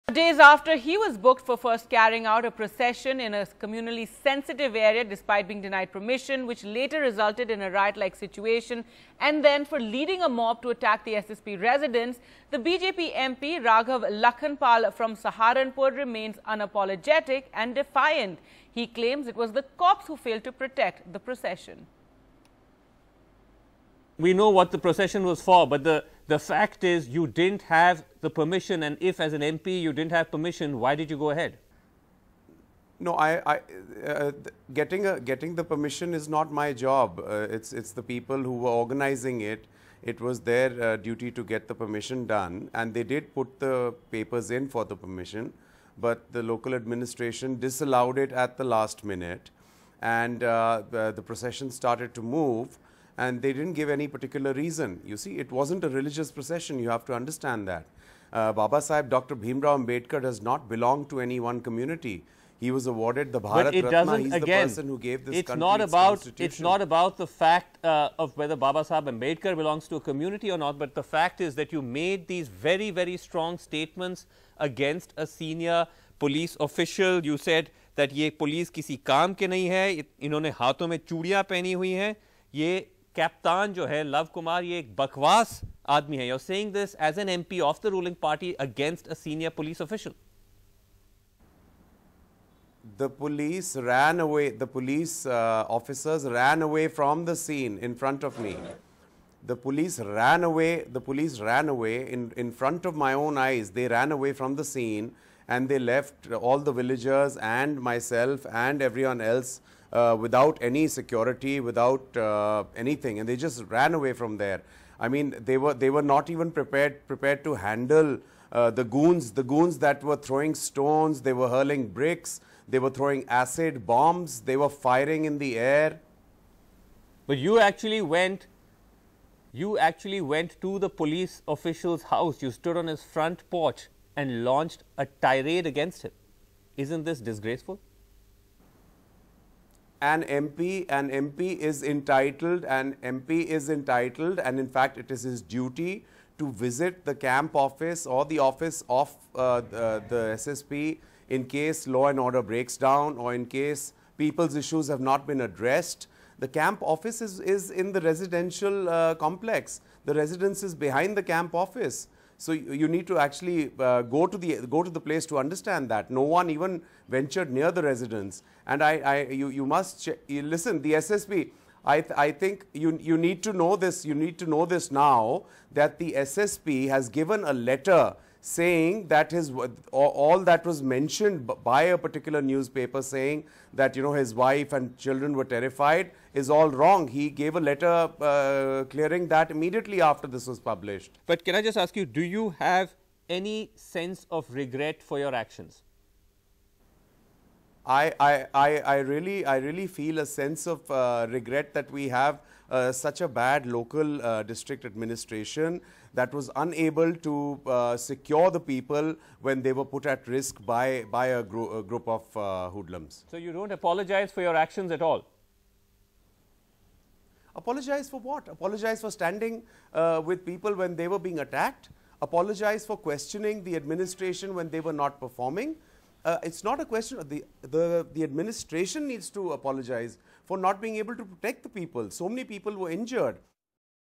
'Apologise, Why?' BJP Leader Accused Of Rioting In UP's Saharanpur Defiant
A BJP MP charged with leading a procession without permission - which resulted in a riot-like situation in a village in Uttar Pradesh's Saharanpur - and then targeting a senior police officer's home with a mob, is unrepentant. "Apologise for what?" a defiant Raghav Lakhanpal told NDTV.